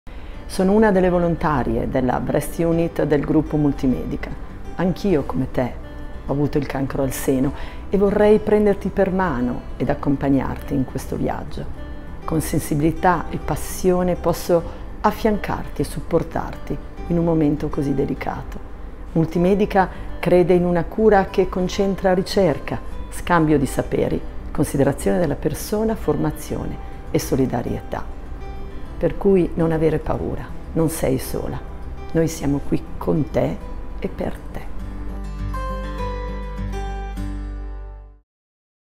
nonaverepaura - Il messaggio di Adele Patrini
Ti prendiamo per mano in ogni fase del percorso di cura.
Questo è uno dei messaggi che la Breast Unit MultiMedica desidera veicolare grazie alla nostra ambasciatrice, Adele Patrini, Presidente dell'Associazione C.A.O.S. Onlus.